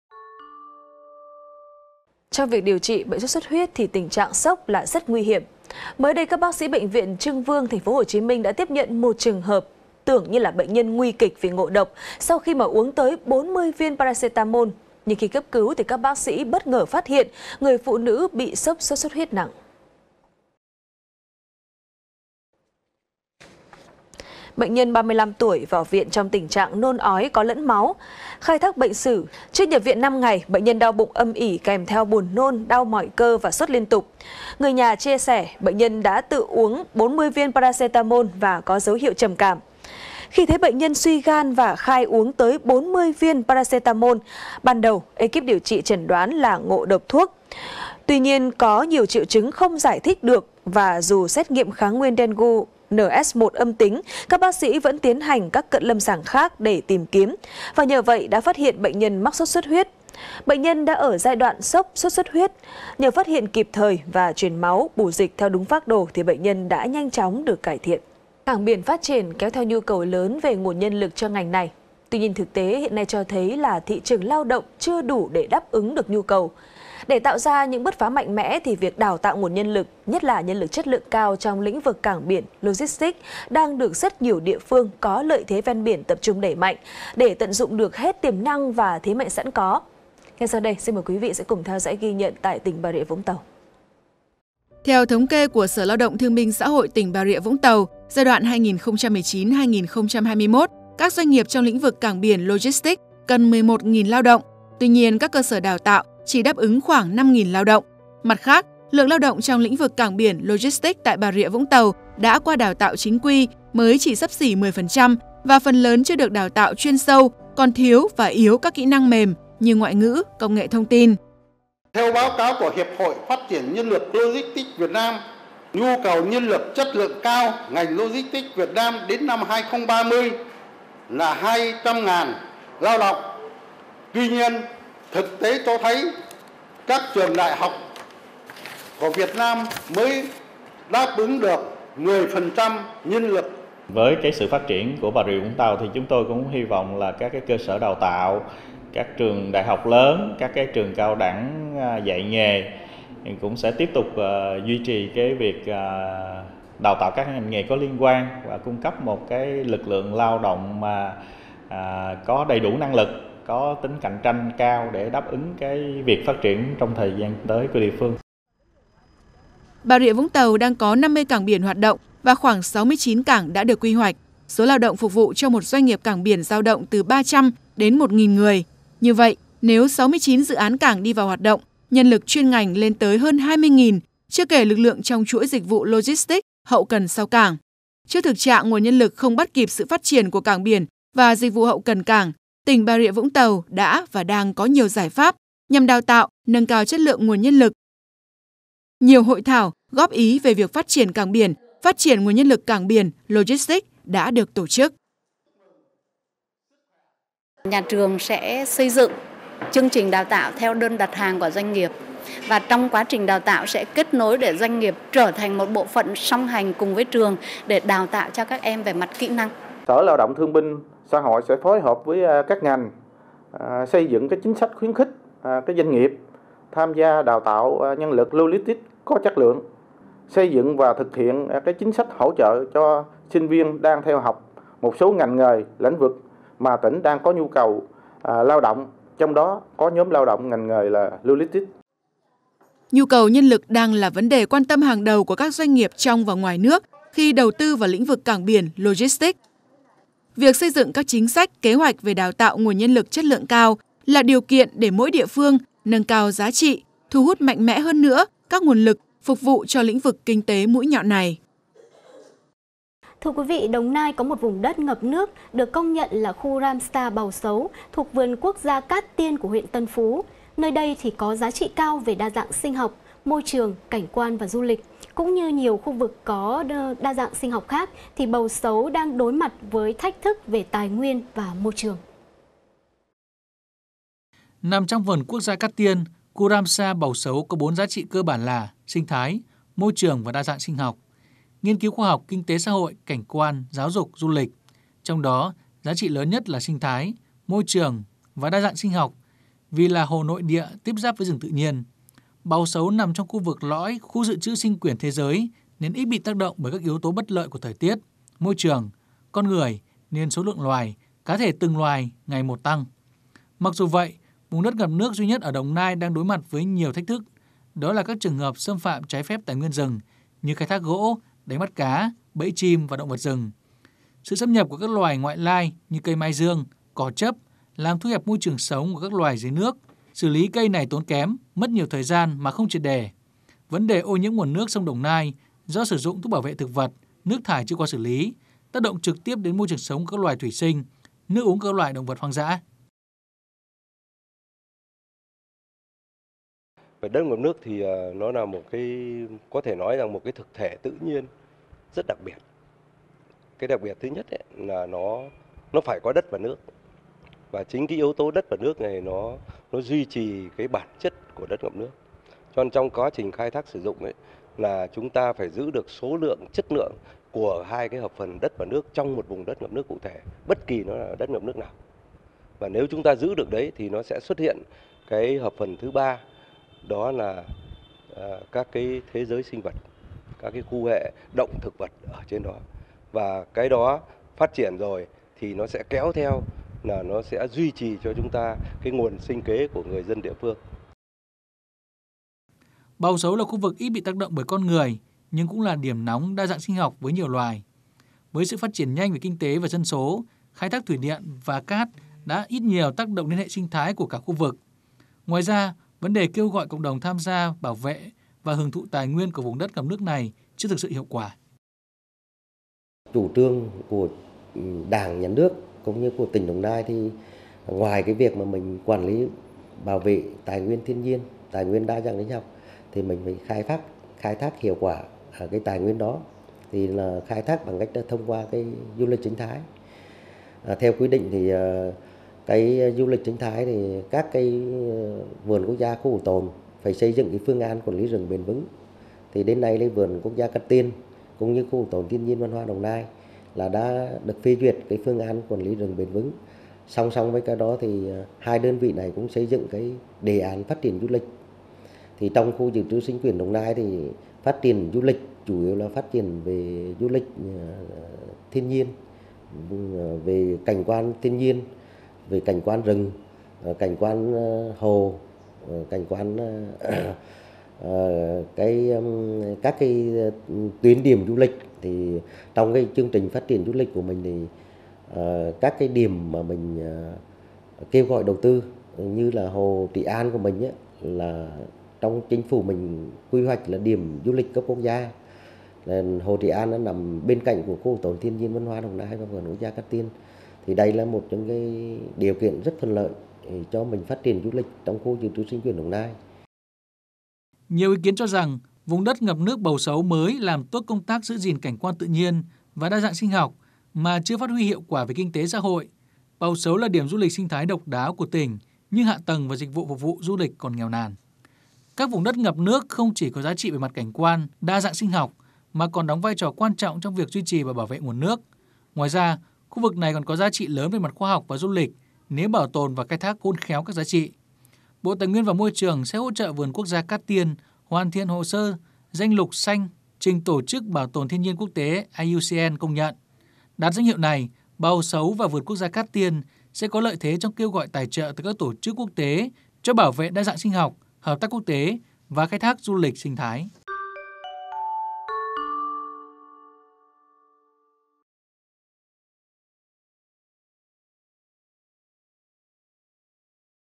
TPHCM: Người phụ nữ bị sốt xuất huyết nguy kịch vì tự ý uống 40 viên paracetamol | VTC Tin mới
VTC Tin mới | Ngày 29/11, Bệnh viện Trưng Vương (TPHCM) cho biết, các bác sĩ mới đây kịp thời cứu sống một người phụ nữ bị sốt xuất huyết nặng, tự ý uống 40 viên paracetamol và nhập viện trễ.